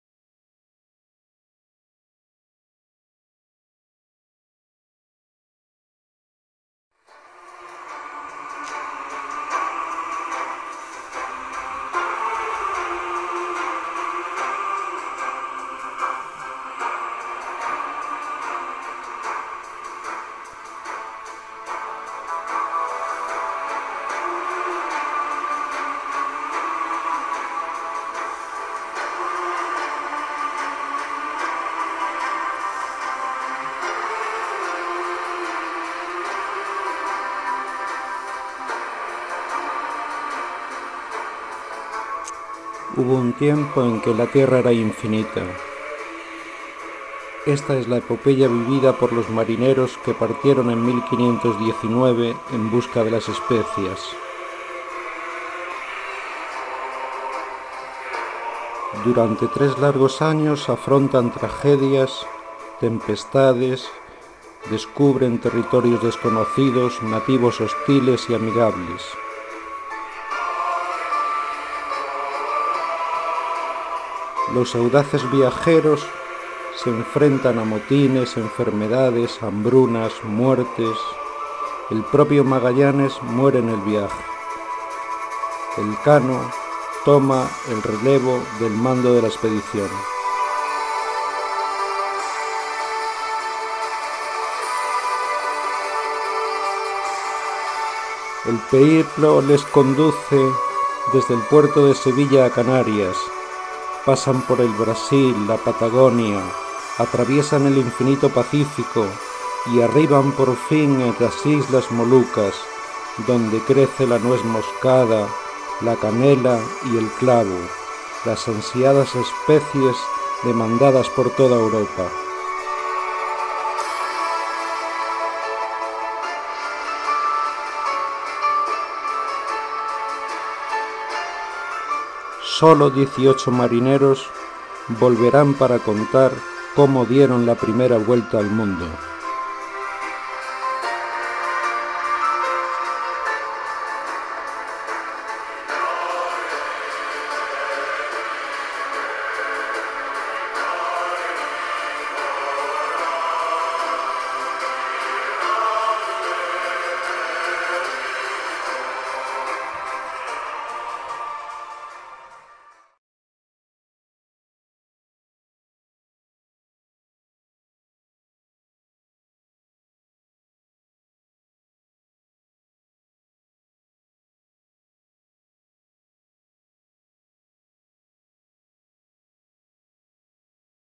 Magallanes y Elcano / Isidoro Castaño
La epopeya de los marineros que dieron la primera vuelta al mundo.